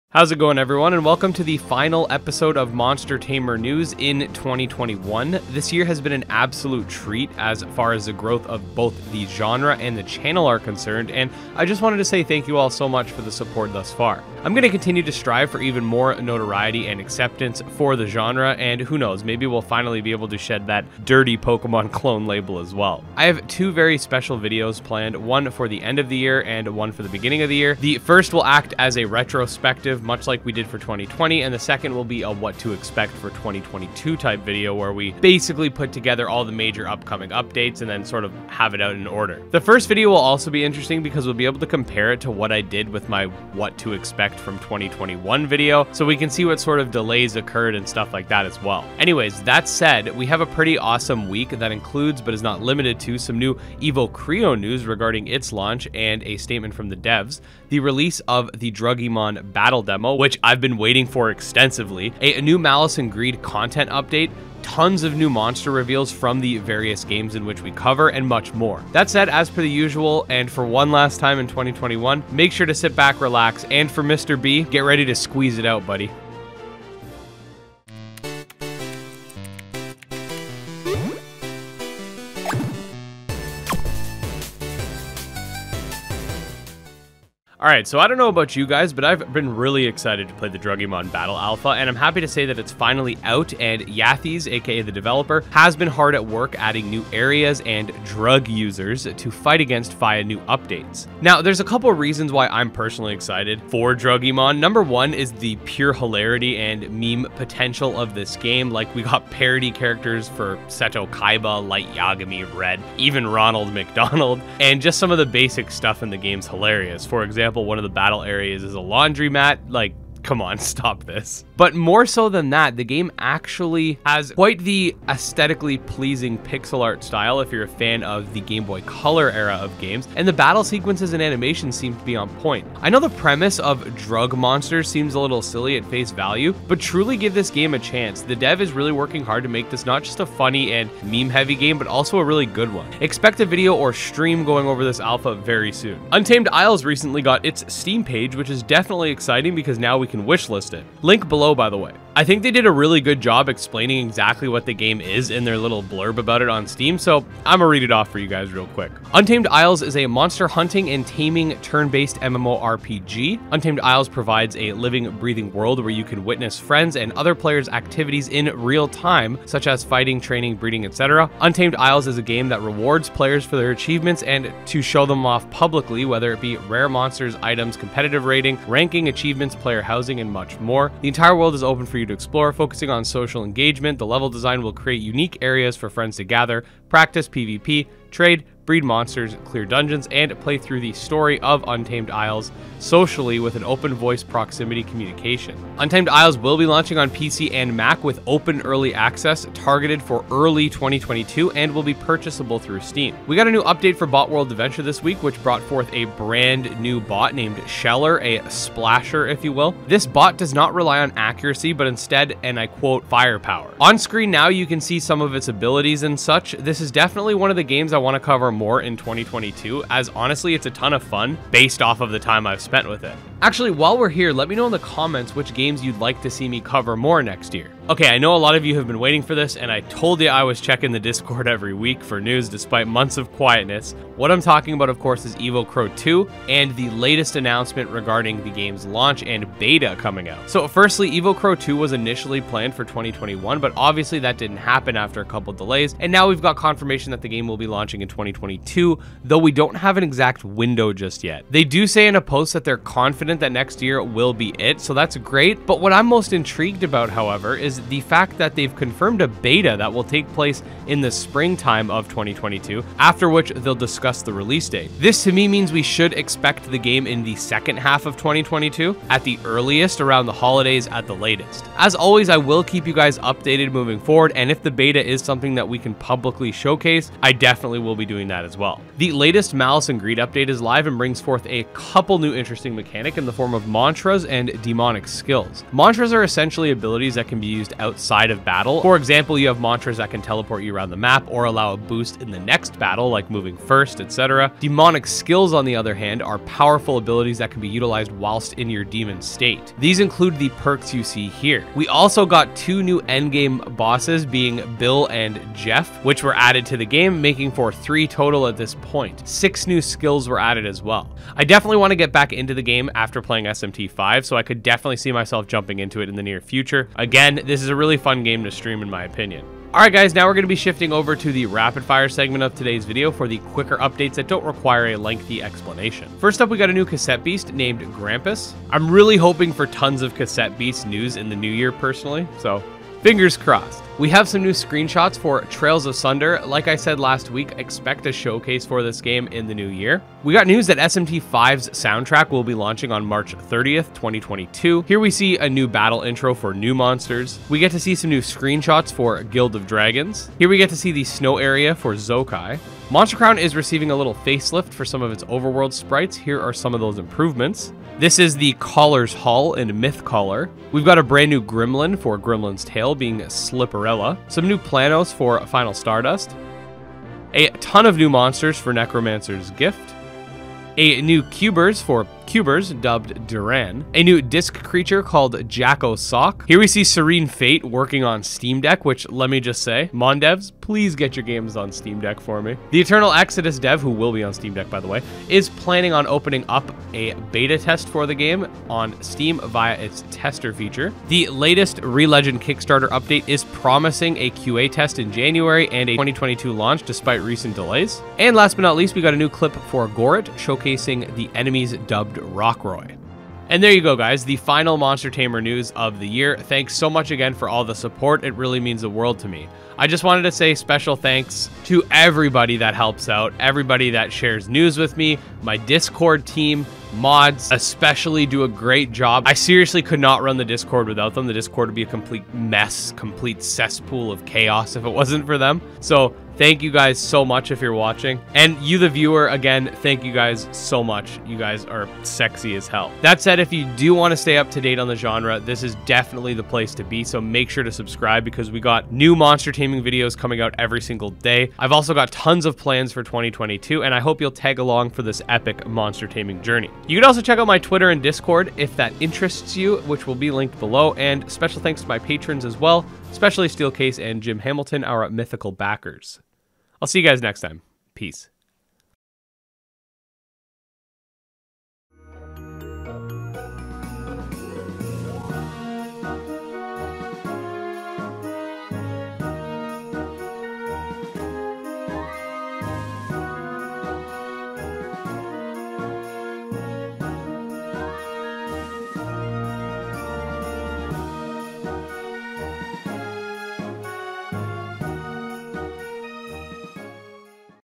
Monster Tamer News: New EvoCreo Release Info and Beta, Druggiemon Alpha, Botworld Update and More!
This week we have a ton of interesting news, including some information regarding Evocreo's release window, and a beta coming in 2022, the release of the Druggiemon Battle Demo, which I'm HYPED for, a new update to Malice and Greed and Botworld Adventure and More!

Legendary Patrons:
-Jim Hamilton

Games Mentioned in Order:

-Druggiemon
-Untamed Isles
-Botworld Adventure
-EvoCreo 2
-Malice & Greed

Rapid Fire News:

-Cassete Beasts
-Trails of Sunder
-SMT 5
-Nuumonsters
-Guild of Dragons
-Zokai
-Monster Crown
-Myth Caller
-Grimmlins Tale
-Final Stardust
-Necromancer's Gift
-Kubberz
-Disc Creatures
-Serin Fate
-Eternal Exodus
-Re:Legend
-Gorit

Avatar: Bisloba